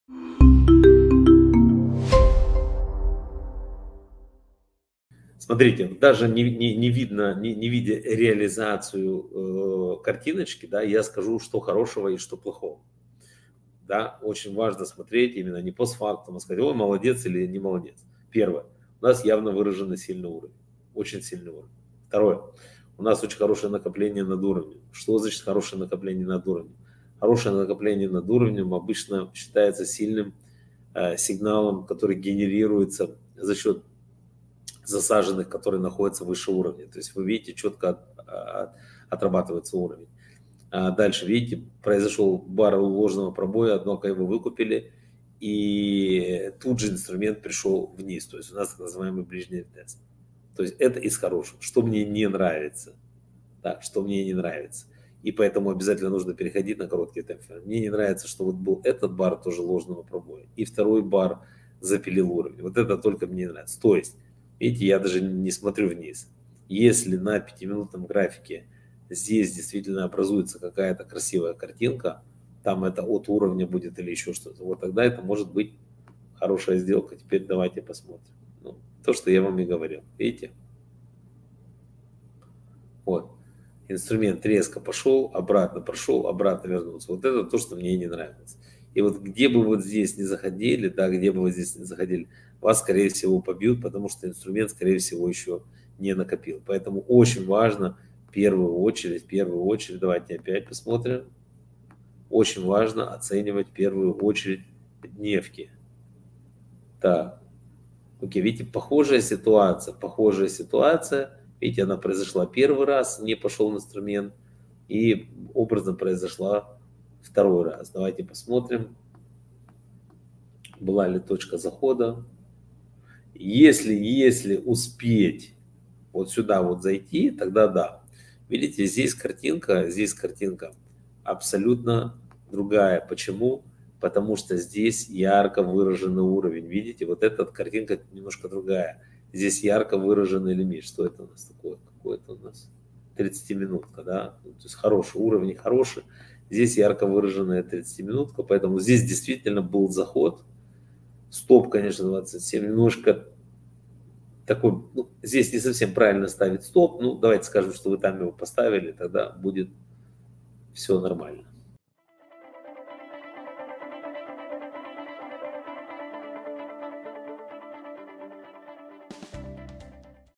Анализируем сделки ПОДПИСЧИКОВ: Как ПРАВИЛЬНО оценить ТОЧКУ ВХОДА?
Благодаря разбору сделок трейдеров с валютами и акциями, анализу ошибок трейдеров с Александром Герчиком вы сможете совершенствовать свою торговую систему и быть увереннее в своём трейдинге. Ваша сделка может попасть на разбор к Шефу! Присылайте ее сюда ➡️